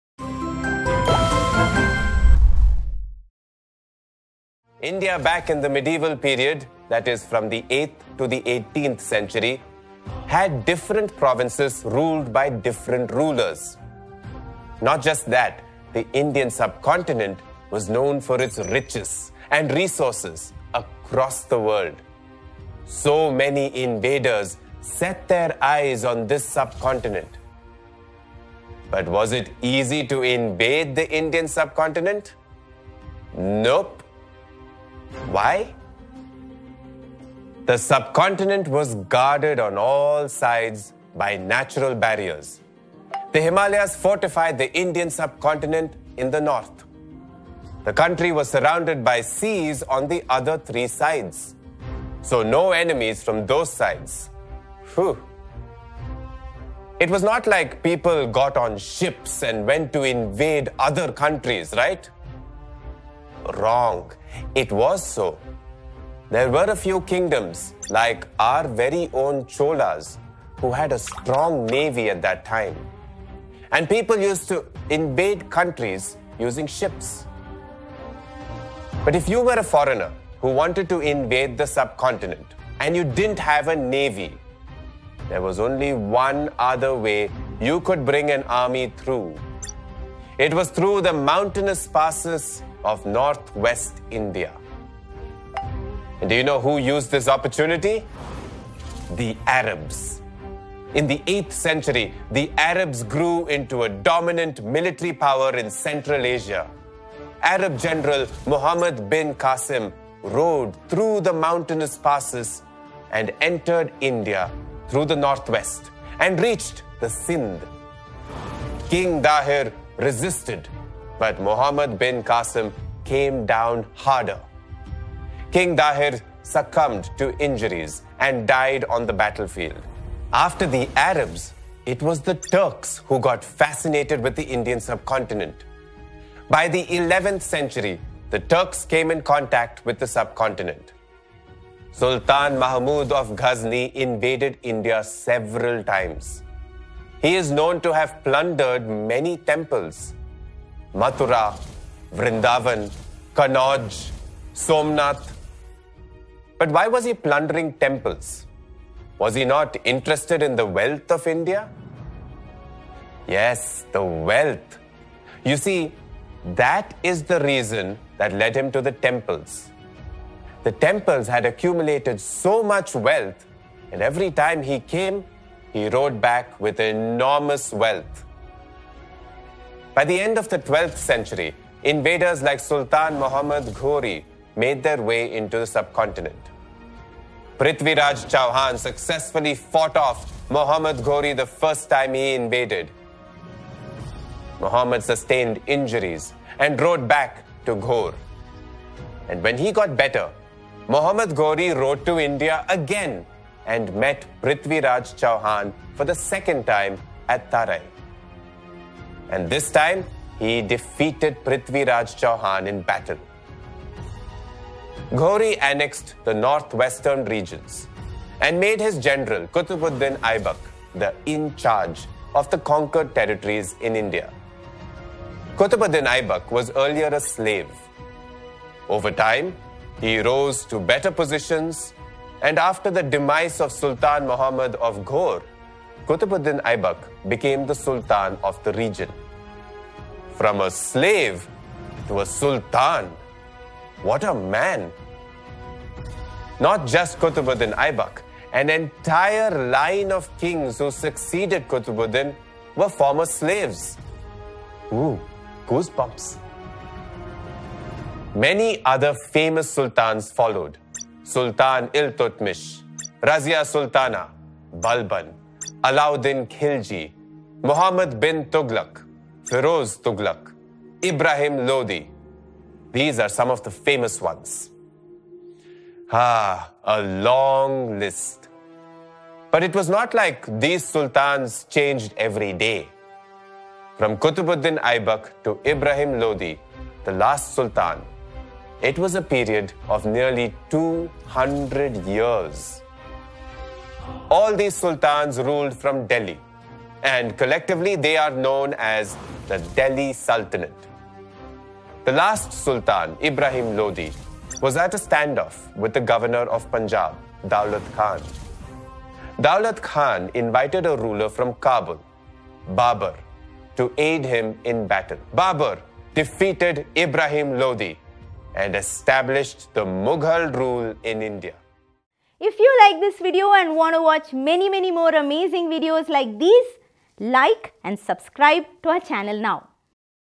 India During The Medieval Era | Class 7 - History | Learn With BYJU'S
Medieval India (8th - 18th Century) had different provinces ruled by different rulers. Known for the riches and resources, India attracted many foreign invaders. But the geography of our land made it very difficult for traders and invaders to enter. How was India protected by natural resources? Watch this video to find out!

💸 Cash Rewards
🏆 Be an All-India Rank